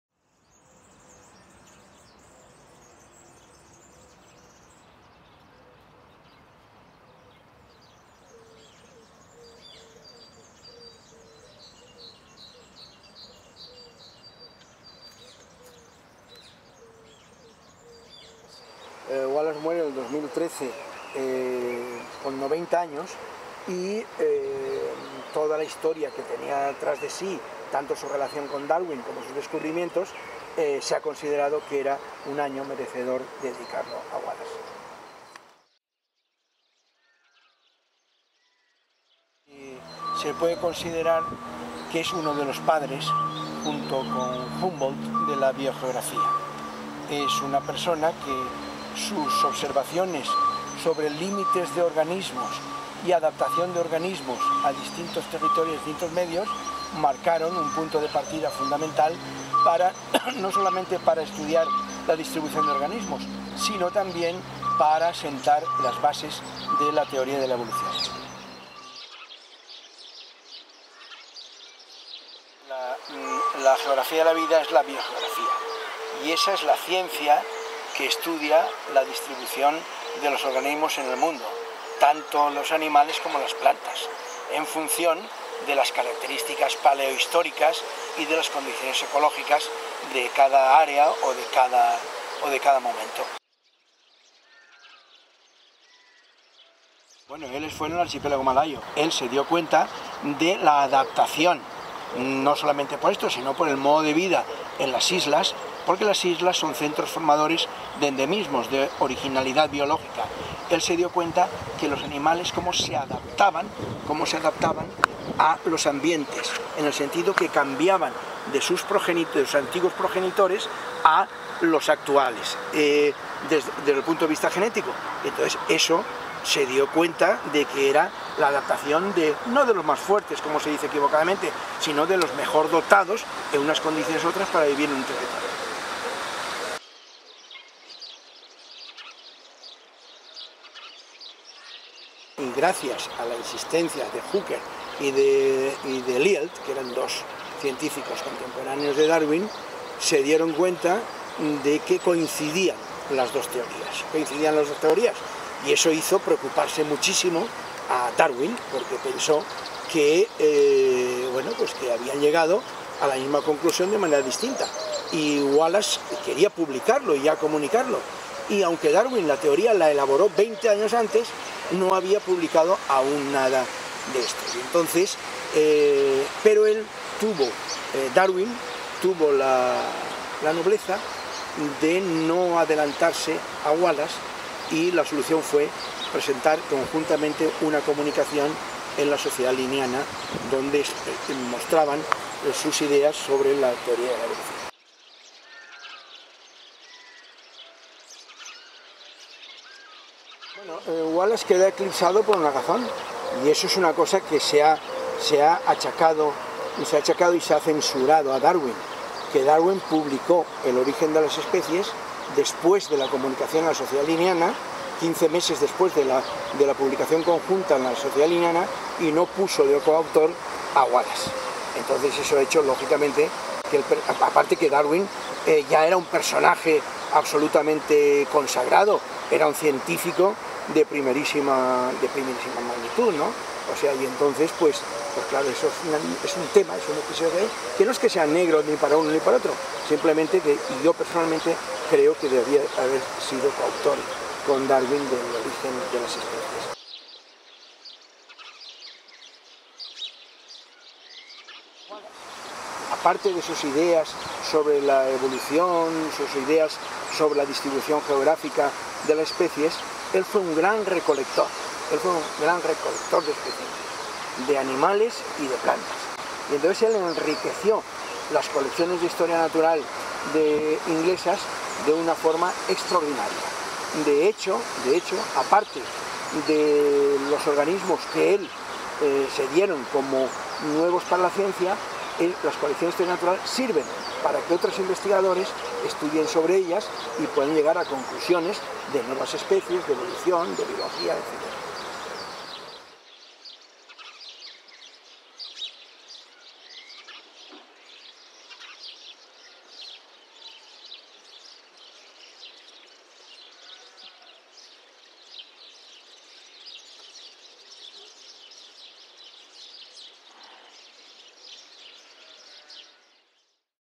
Entrevista a Manuel Costa
Manuel Costa, Catedràtic de Botànica de la Universitat de València, ens parla de la figura d'Alfred Russel Wallace. L'any 2013 ha estat declarat por la comunitat científica com a Any Wallace. Amb motiu d'aquesta commemoració la Universitat va organitzar un cicle anomenat «La geografia de la vida».